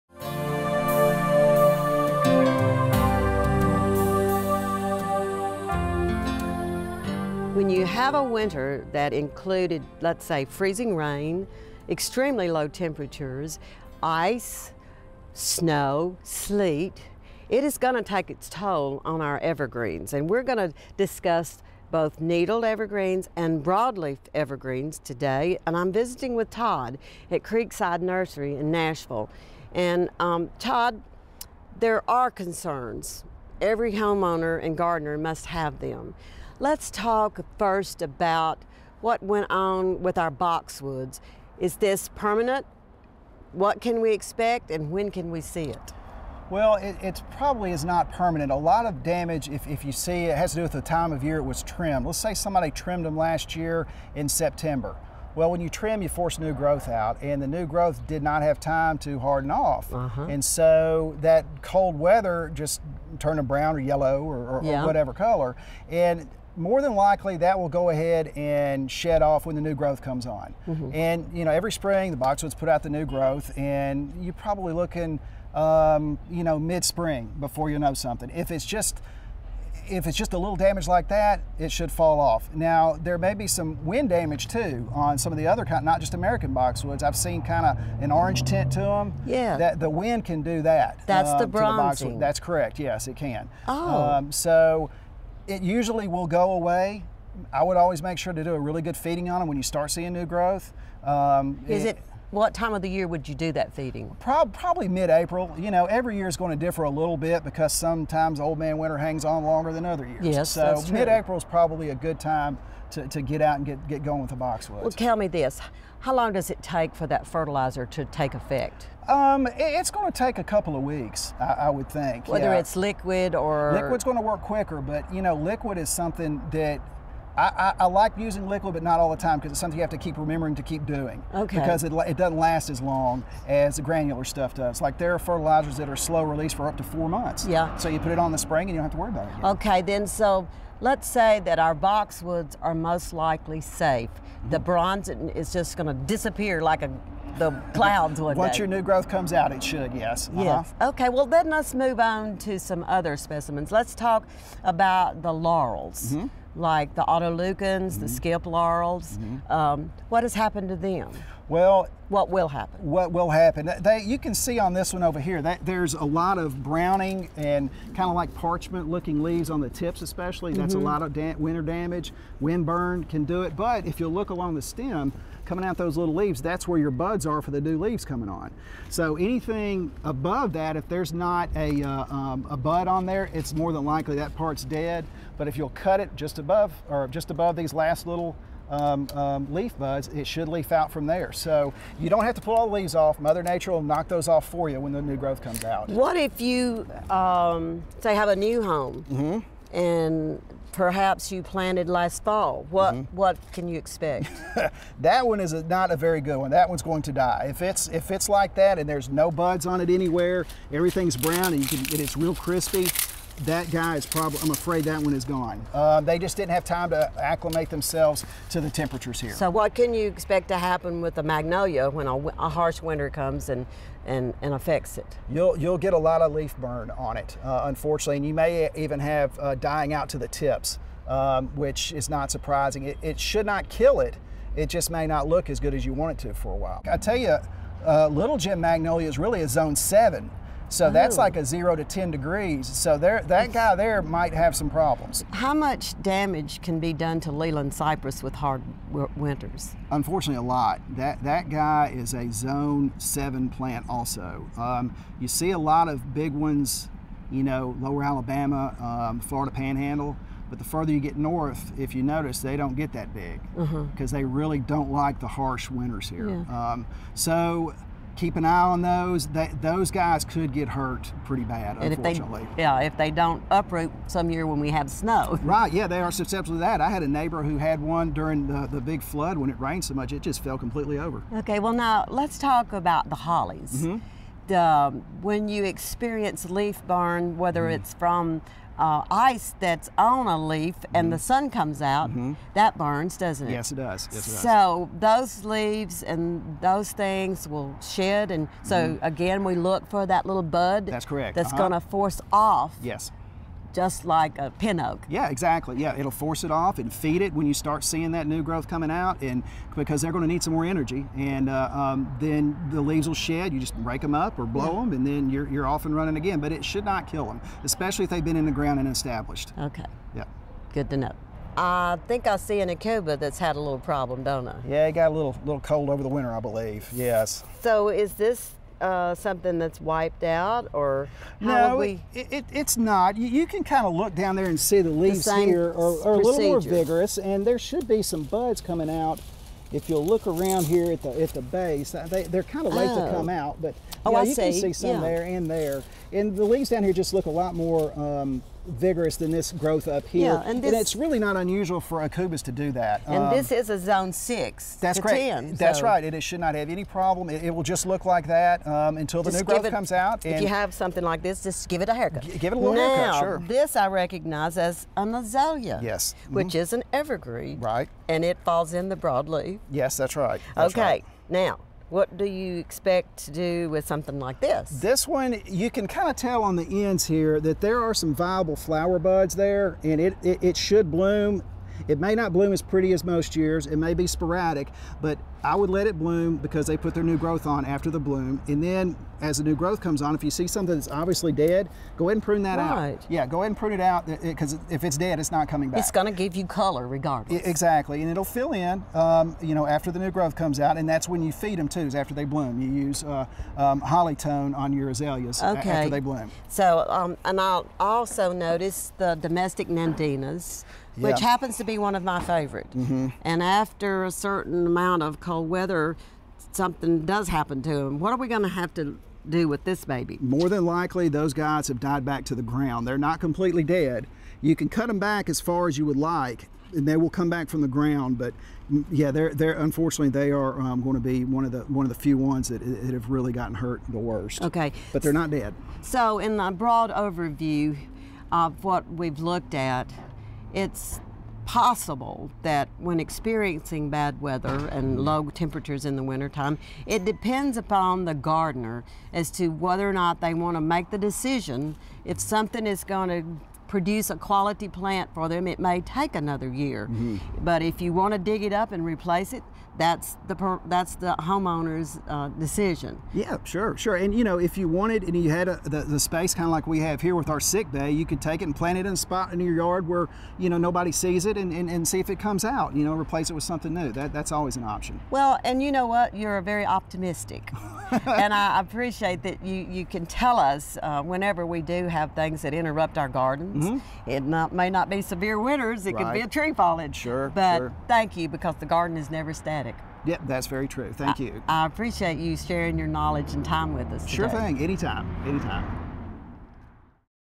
Evergreen Bronzing | Volunteer Gardener
Some evergreen shrubs and trees have suffered during the low temperatures and frozen precipitation of winter, as evidenced in the bronzing of leaves and needles.  We'll take a closer look at the damage and get recommendations for these staples of the garden.